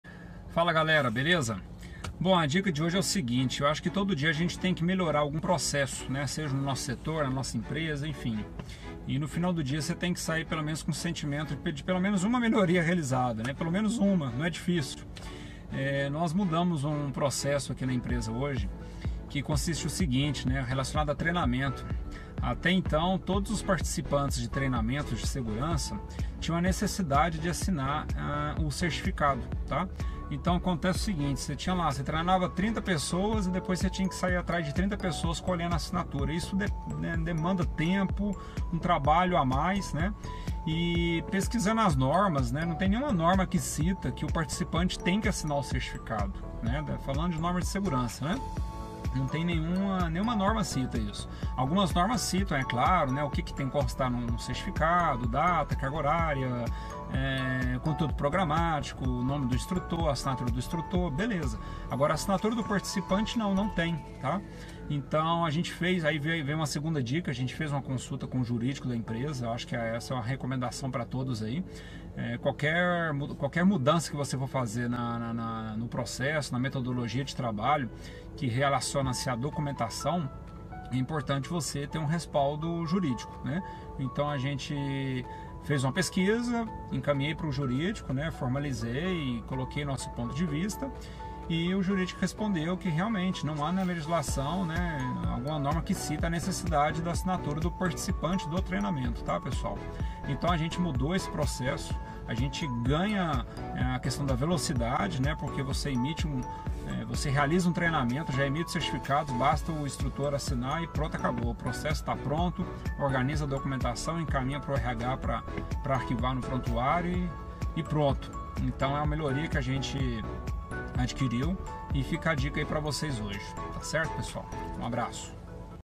DICA DE SST #4: ASSINATURA DO PARTICIPANTE NOS CERTIFICADOS DE TREINAMENTOS DE SST
Para implementar essa dica é importante que você analise se os procedimentos internos da empresa não exige a assinatura dos participantes nos certificados.

Nas normas regulamentadoras não há essa exigência, o que facilita e muito na produtividade do nosso setor onde temos inúmeras demandas.

São atitudes simples assim que economiza um grande tempo na rotina do setor de SST.